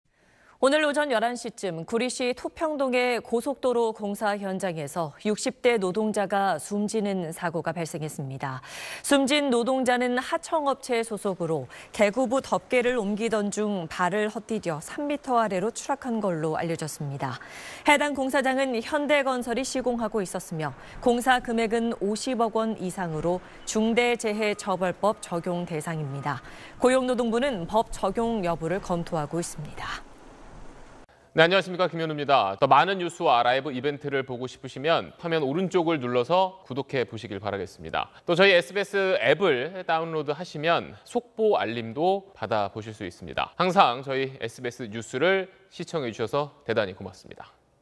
구리 토평동 고속도로 공사현장에서 근로자 1명 추락사 / SBS
오늘(16일) 오전 11시쯤, 구리시 토평동의 고속도로 공사 현장에서 60대 노동자가 숨지는 사고가 발생했습니다.
이 노동자는 하청업체 소속으로 개구부 덮개를 옮기던 중 발을 헛디뎌 3m 아래로 추락한 것으로 알려졌습니다.
해당 공사장은 현대건설이 시공하고 있었으며, 공사 금액은 50억 원 이상으로 중대재해처벌법 적용 대상입니다.